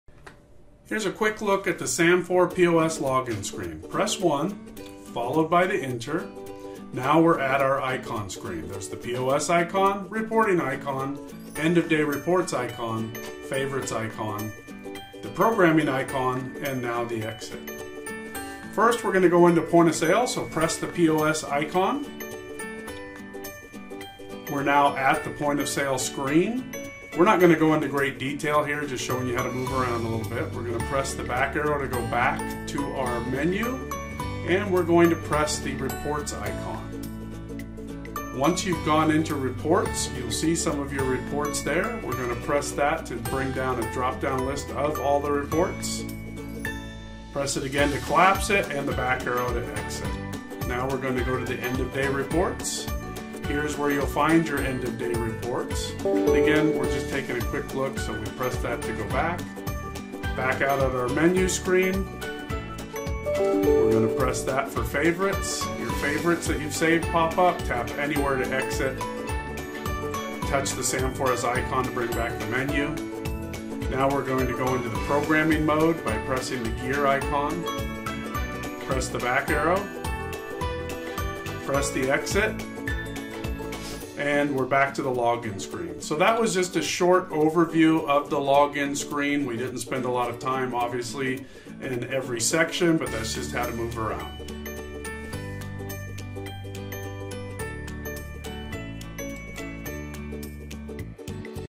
SAM4pos Log In Screen on the SAP-6600 and SAP-630
Here we are looking at the basic log in screen and how to move around into and out of the different modes. We are using the default log in of #1 , but for your business that has probably been changed.